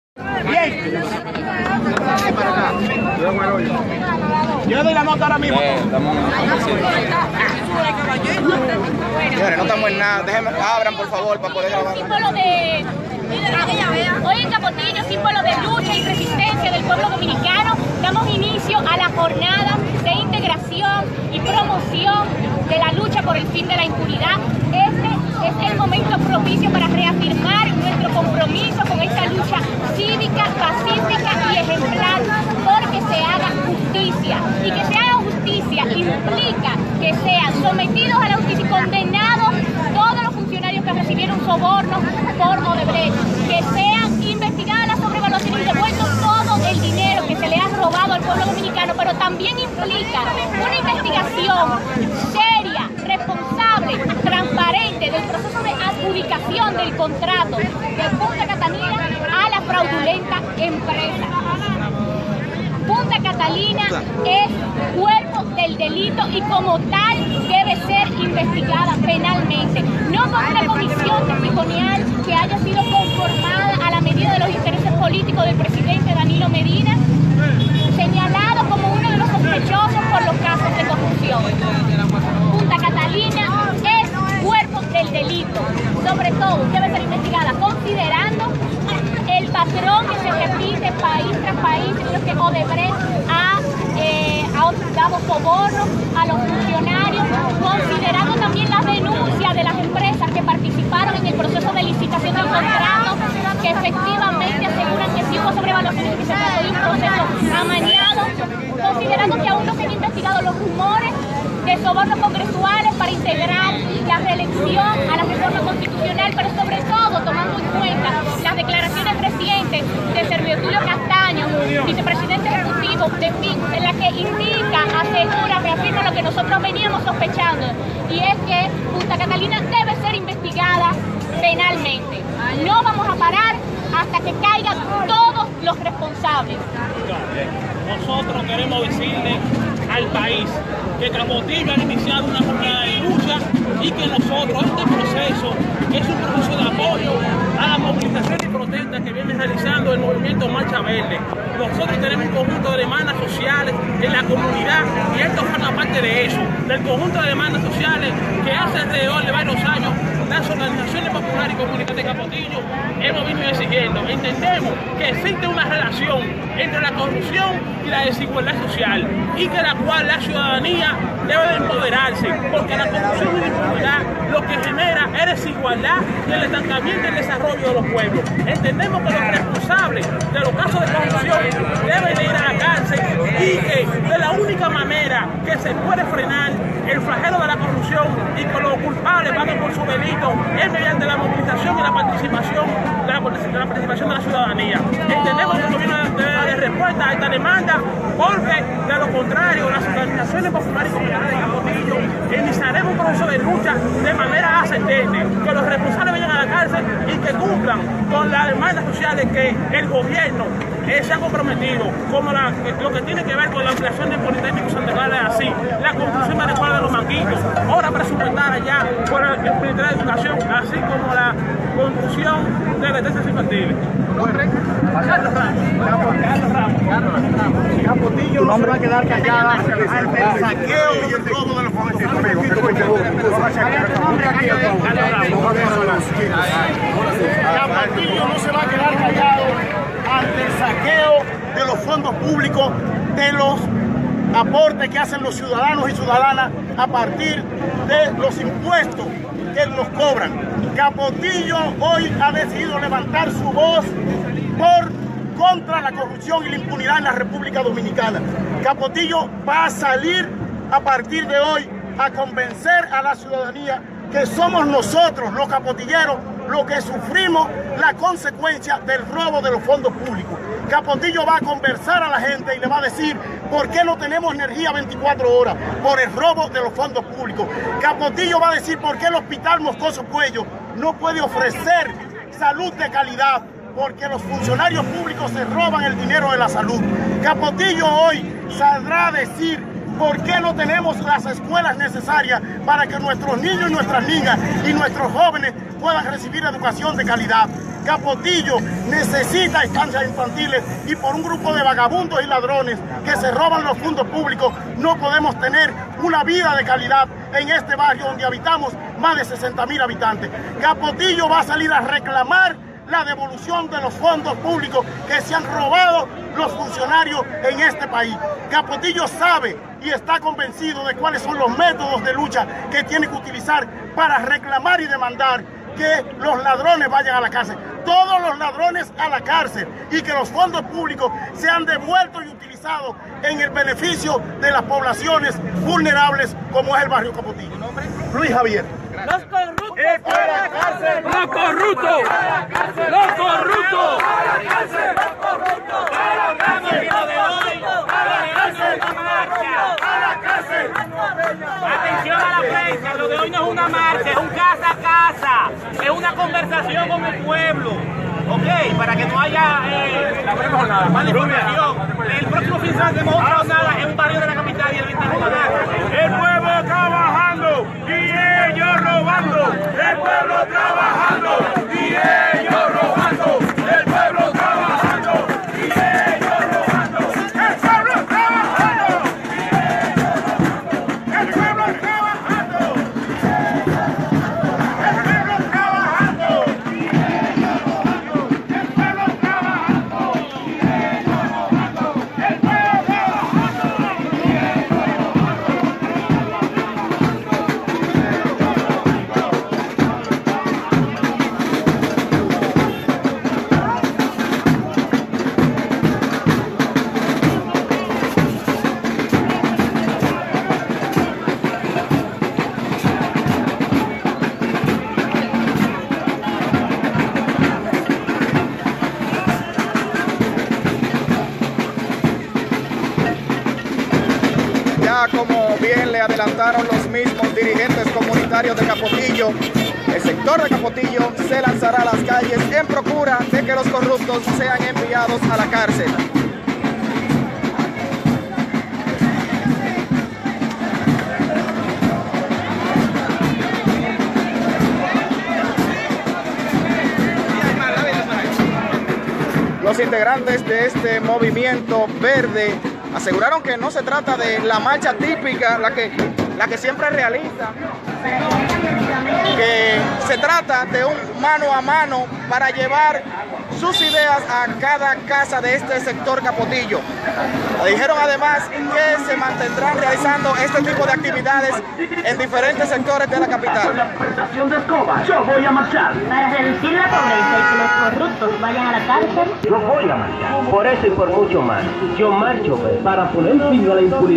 Manifestantes del Movimiento Verde realiza jornada barrial en Capotillo.
Por: Joan Vargas 14/05/2017